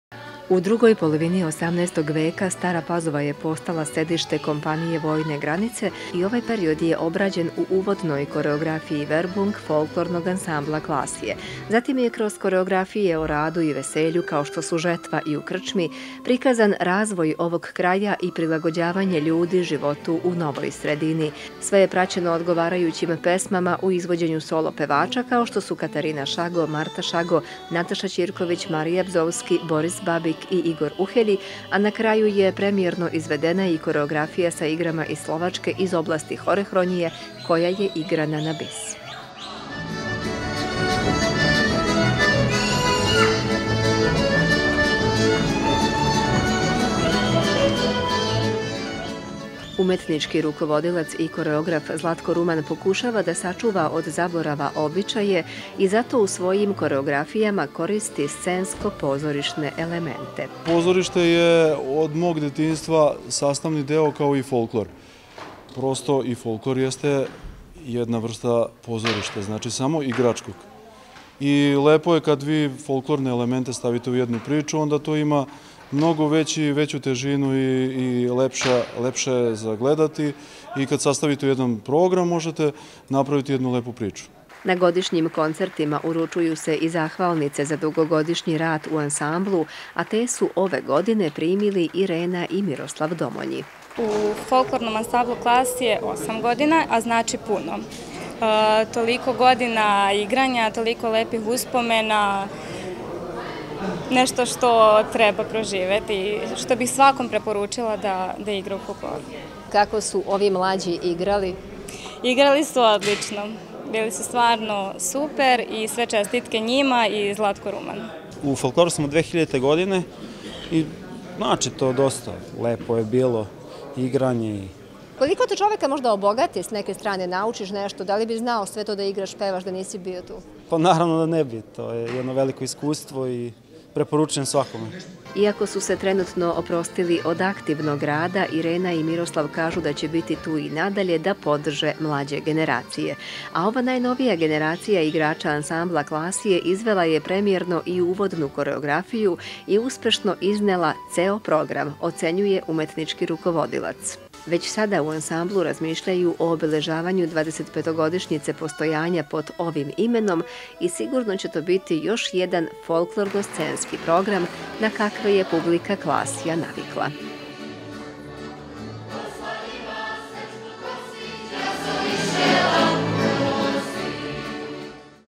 24. godišnji koncert FA Klasje
Za svoj 24. godišnji koncert  FA Klasje SKUD-a Janko Čmelik iz Stare Pazove pripremio je folklorno – muzičku priču o naseljavanju Stare Pazove i o životu Slovaka na ovim prostorima.